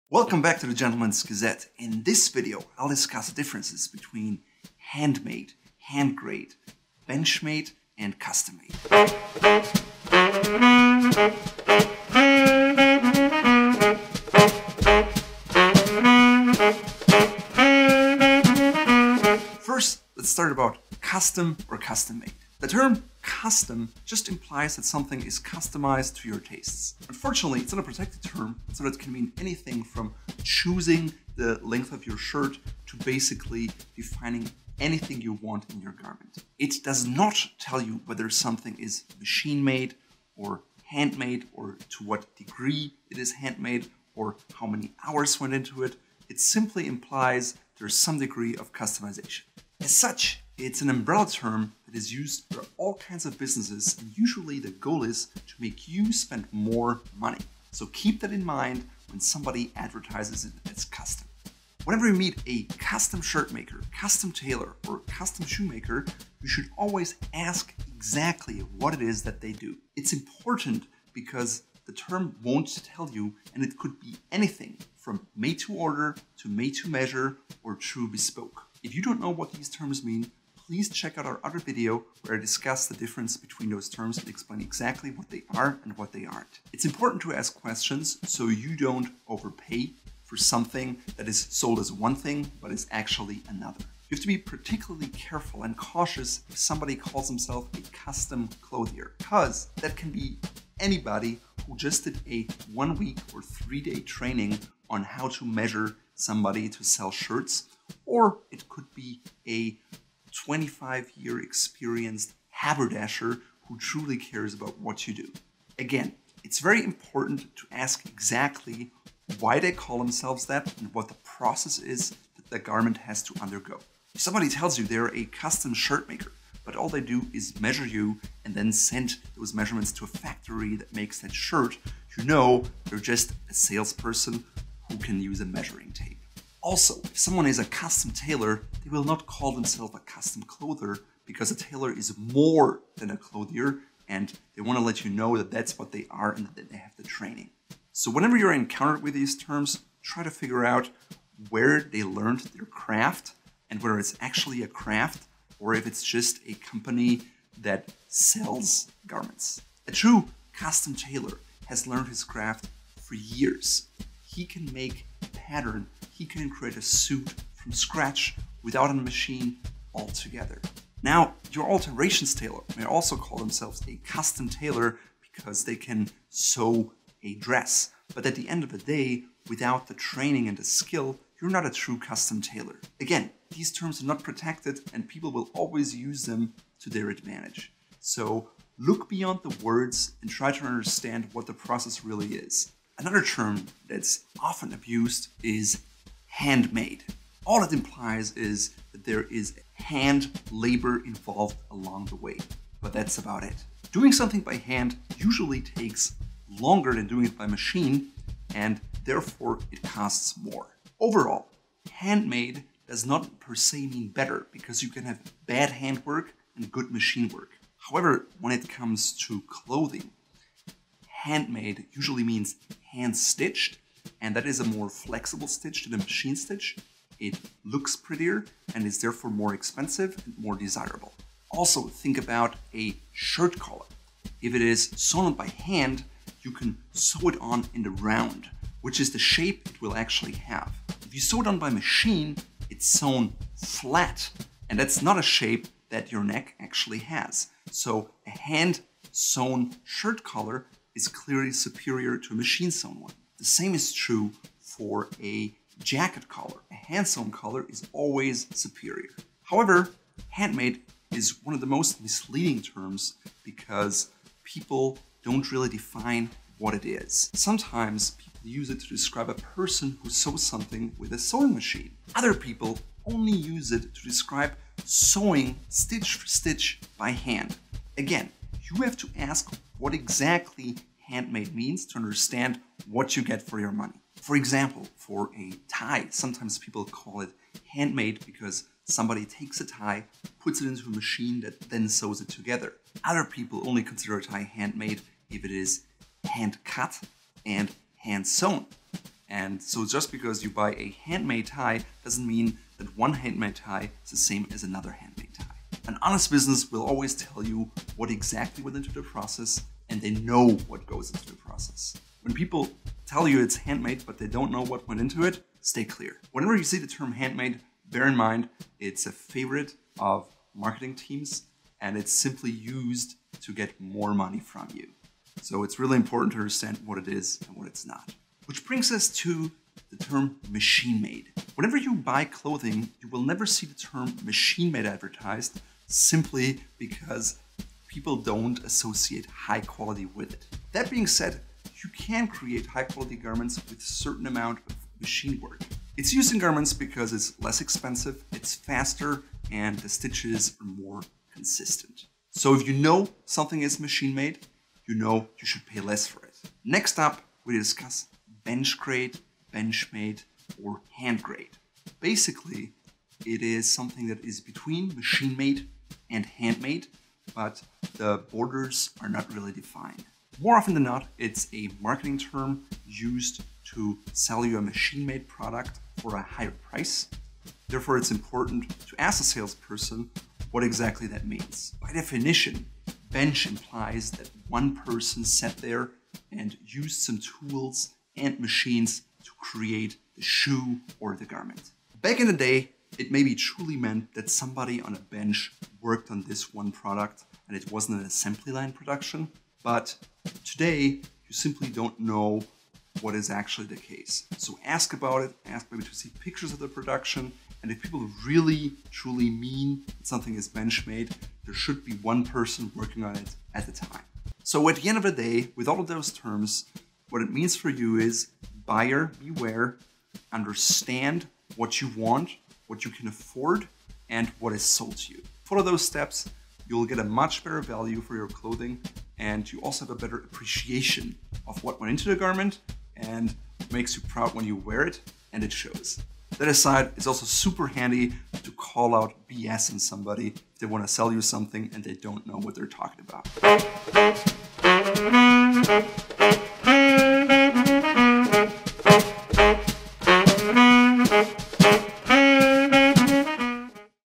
Custom Made, Handmade & Bench Made - Suits, Shoes, Shirts Differences & Terminology Explained
Familiarize yourself with these common terminologies in men's clothing so you always know what you're getting and get the most out of your money.

Custom made
- simply means something is customized to your taste.
- basically defining anything you want in your garment.
- Umbrella term used by businesses to get you to spend more money.
- Always ask the right questions. ( What they do and why they call themselves that and the process they have)

Handmade
- Hand labor involved, that's it!
- Usually takes longer than doing it by machine and therefore, it costs more.

Machine made
- Never advertised simply because people don't associate high quality with it.
- Note that if you know something is machine made, you know you should pay less for it.

Bench grade, Bench made or Hand grade
- Basically, it is something that is between machine made and handmade, but the borders are not really defined.
- A marketing term used to sell you a machine made product for a higher price.

Take note, buyer beware! Know and research what you want, what you can afford, and what is sold to you. It pays to have a better understanding of what you're wearing.